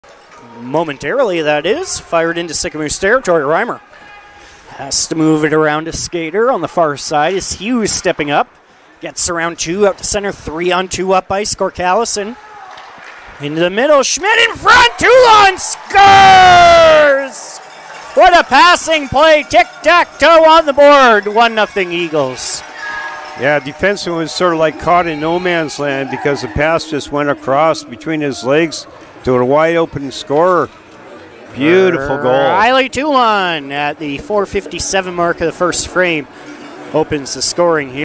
Eagles Goal #1 vs Summerland Steam - Feb 11, 2023
Goal scored by #20 Riley Teulon, assisted by #21 Colton Schmidt and #15 Cortt Callison.